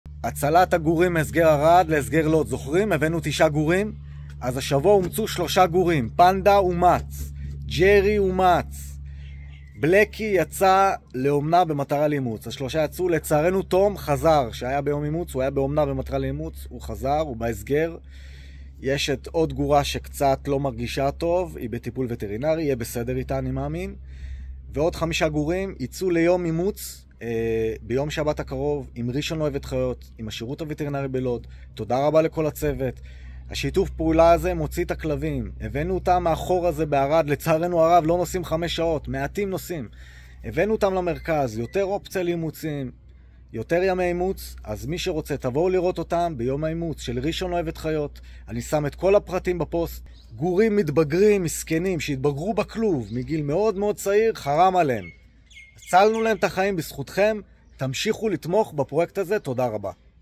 מבצע להצלת גורים מהסגר ערד💙 עדכונים חדשים 10.5.24
השבוע אומצו 3 כלבים שהוצאנו מהסגר ערד :)
ג'רי, פנדה ובלאקי.
לצערנו תום חזר מאומנה
ויש עם תום עוד 4 גורים שיהיו מחר ביום אימוץ
ואנחנו ממשיכים לחפש להם בית.
אפשר לבוא מחר לראות את הכלבים
רושמים בוויז ראשון לציון אוהבת חיות
בין השעות 11-14
אפשר לצור איתי גם קשר בווצאפ לגבי אימוץ הגורים: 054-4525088 רועי
לפרטים נוספים על הפרויקט להצלת הגורים שהוצאנו מערד: